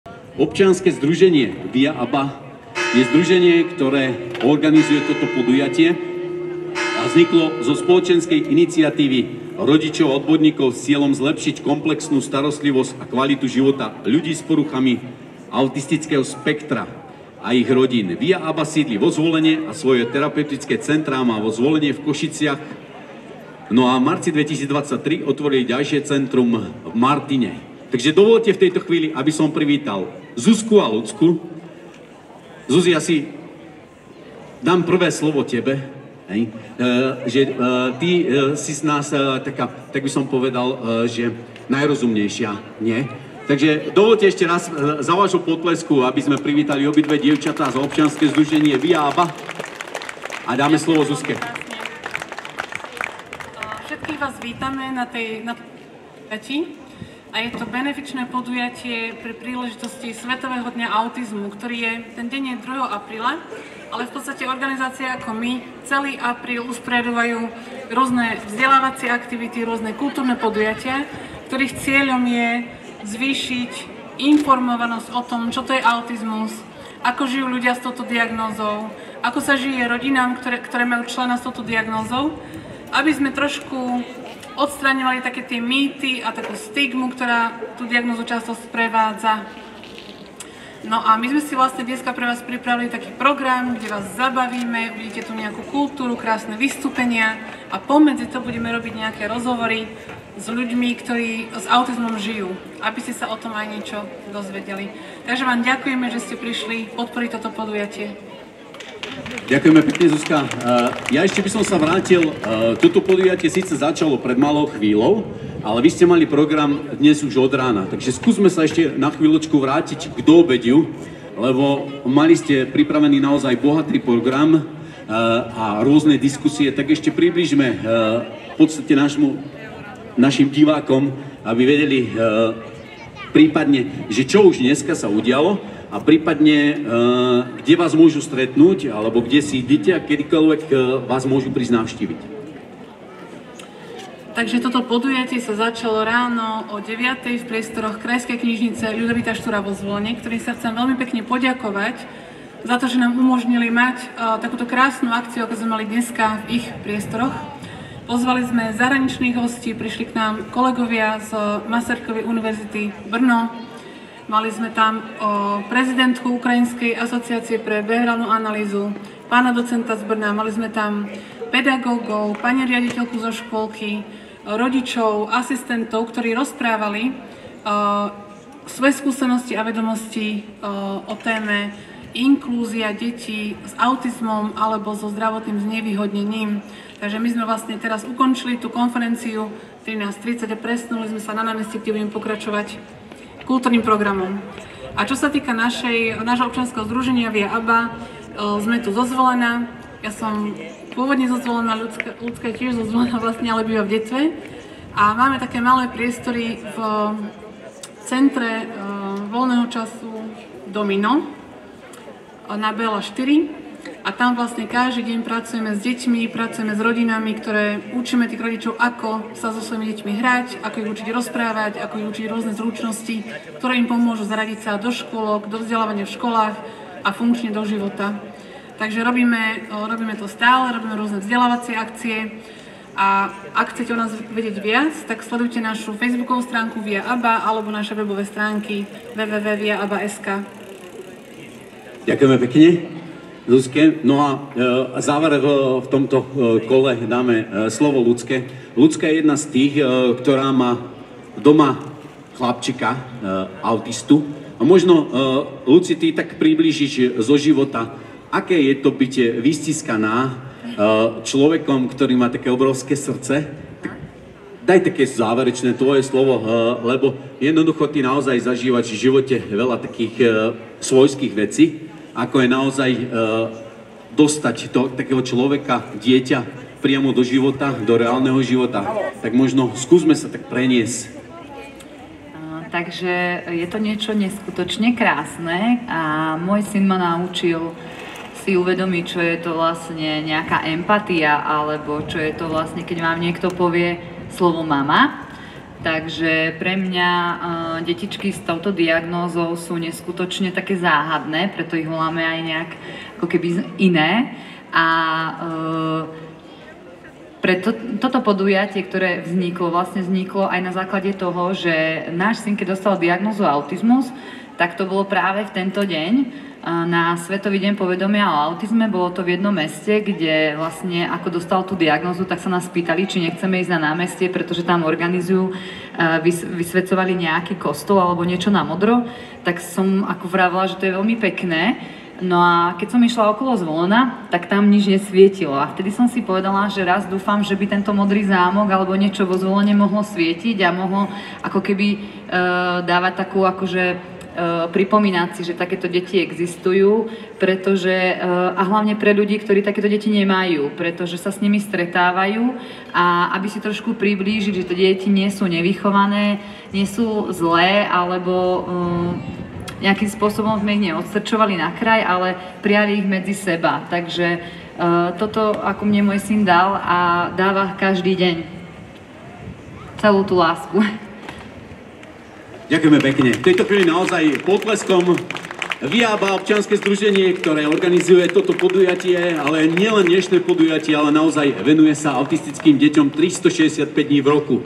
VIA ABA - Vysvieťme Zvolen na modro!
V sobotu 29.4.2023 sa na Zvolenskom námestí konalo podujatie pod názvom Vysvieťme Zvolen na modro!

O VIA ABA prišli niečo viac povedať hlavné organizátorky Zuzana Maštenová a Lucia Lazárová.